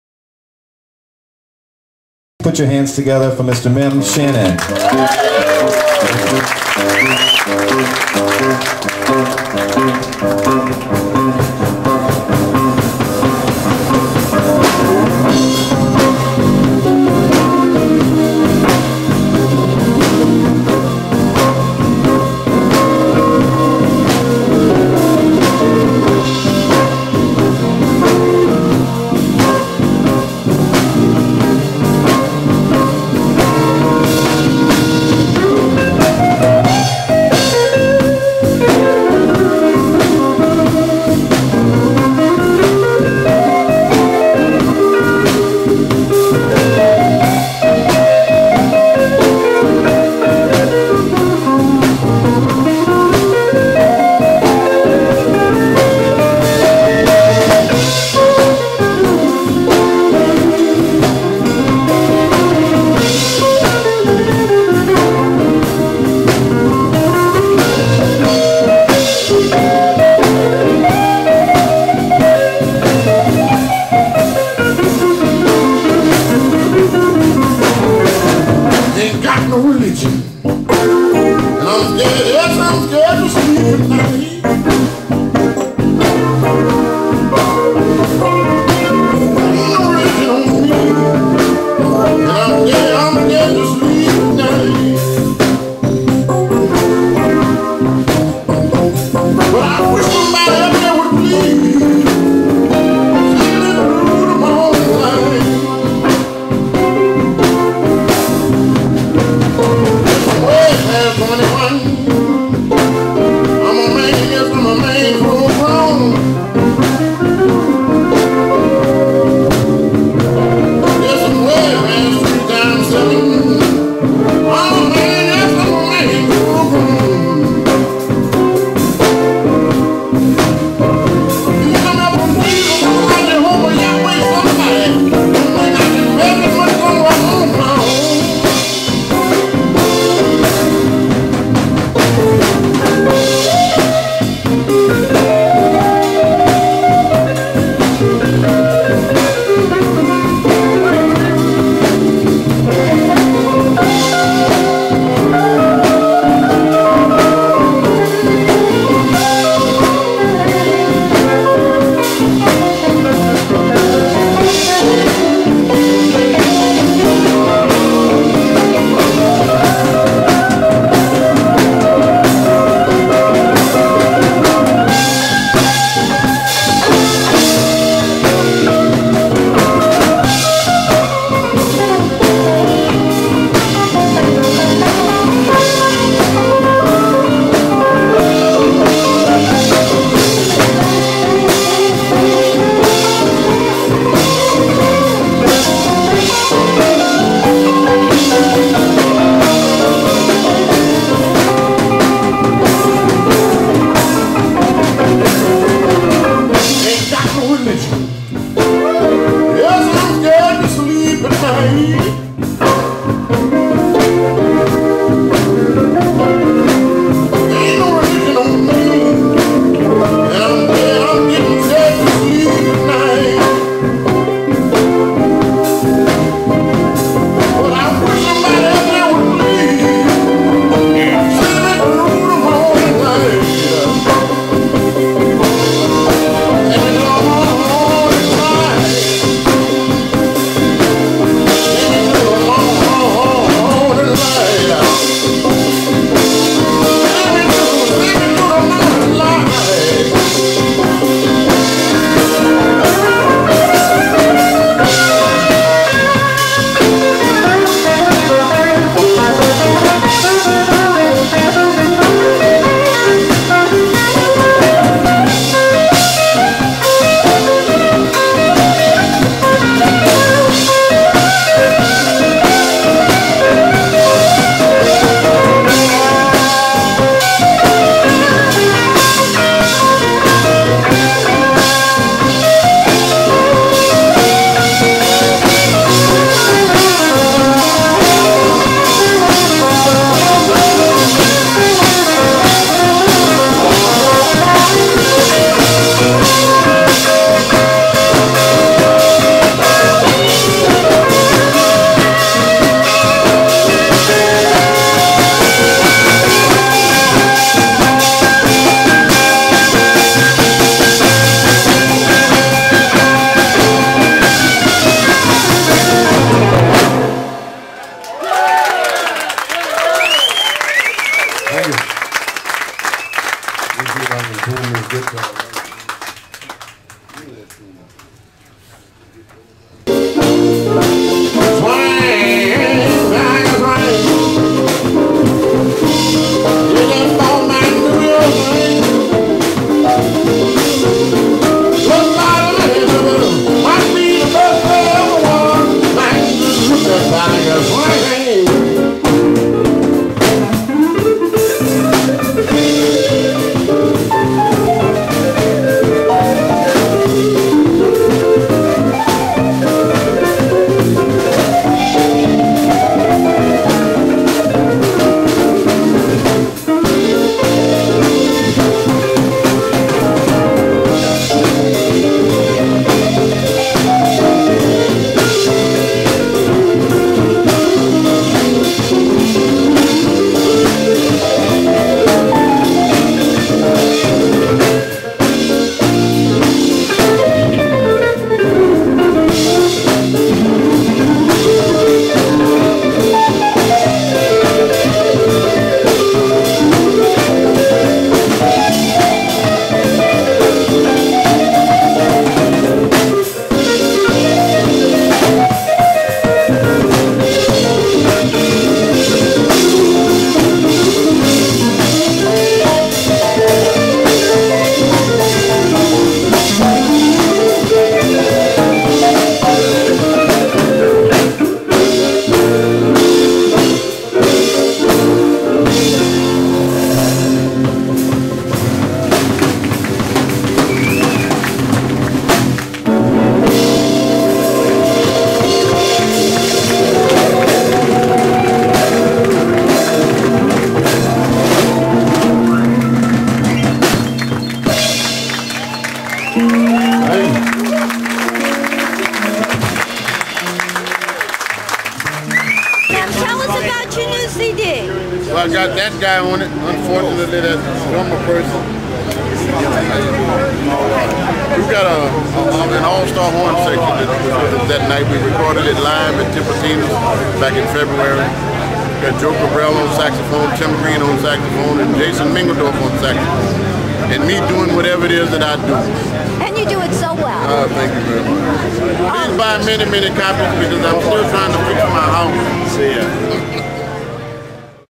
MEM SHANNON @ Louisiana Music Factory JazzFest 2007
Louisiana Music Factory Jazz Fest In-Stores 2007
BLUES - Musical Performance & Interview